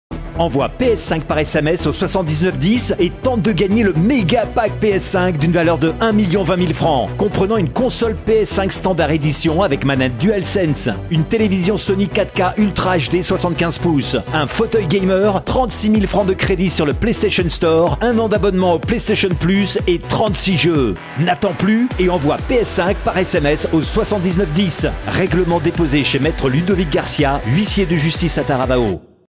TahitiKDO | Tente de GAGNER le MEGAPACK PS5 d'une valeur de 1.020.000 Fcp
🎁 Voilà un KDO qui va ravir tous les Gamers de Polynésie ! TahitiKdo t'offre ce MEGAPACK PS5 d'une valeur de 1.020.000 Fcp 🤩 comprenant:

🖲 1 Console PS5 Standard Edition
➕ 🎮 Manette Dual Sense
➕ 🖥 TV SONY 75' (190cm) 4K Ultra HD KD-75XH9096
➕ 🎶 Barre de son SONY HT-ZF9 Wifi & Bluetooth
➕ Fauteuil Gamer avec repose pieds NOBLECHAIRS EPIC serie
➕ 3️⃣0️⃣0️⃣€ d'achat sur le Playstation Store

➕ 3️⃣6️⃣ Jeux PS4/PS5

👉 Pour tenter de GAGNER ce MEGAPACK PS5 d'une valeur de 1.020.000 Fcp rien de plus simple, envoie 𝗣𝗦𝟱 par sms au 7️⃣9️⃣1️⃣0️⃣*.

Fin du jeu le 08/02/21, Tirage au sort le 09/02/21. Bonne chance ! 🤞🍀

✔️ Liste des 36 jeux offerts dans le MEGAPACK PS5:
DEMON'S SOULS PS5 VF
SPIDER-MAN MILES MORALES PS5 VF
SPIDER-MAN MILES MORALES ULTI PS5
SACKBOY A BIG ADVENTURE! PS5 VF
UBISOFT Assassin's Creed Odyssey PS4 VF
TAKE2INTERACTIVE WWE 2K19 PS4 VF
ACTIVISION Call Of Duty 15 Black OPS 4 PS4 VF
LEGO DC SUPER-VILAINS PS4 VF
JUST DANCE 2019 PS4 VF
RED DEAD REDEMPTION 2 PS4 VF
RIDE 3 PS4 VF
FALLOUT 76 PS4 VF
BATTLEFIELD 5 P4 VF
F1 2018 P4 VF
SONIC Mania Plus PS4
Sims 4 PS4
DETROIT P4 VF
FARPOINT VR PS4 VF
RIGS VR PS4 VF
The persistence VR PS4 VF
Bravo team VR P4 + AIM controle
Firewall Zero hour vr PS4 VF
Uncharted 4 hits PS4 VF
Rayman Legends Hits PS4 VF
CRASH Bandicoot Trilogy 2.0 P4 VF
V-Rally4 PS4 VF
JUST CAUSE 4 EDITION RENEGAT PS4
PERSONA 3 DANCING MOONLIGHT PS4
PERSONA 5 DANCING STARLIGHT PS4
PLAYER BATTLEGROUNDS PS4 VF
Kingdom Hearts 3 PS4 VF
ACE Combat 7 P4 VF
RESIDENT EVIL 2 PS4 VF
DEAD OR ALIVE 6 Ps4 vF
DARK SOULS TRILOGY Ps4 VF
* Prix du sms 585 Fcp TTC.
** Règlement déposé chez Me Ludovic GARCIA, huissier de justice à Taravao.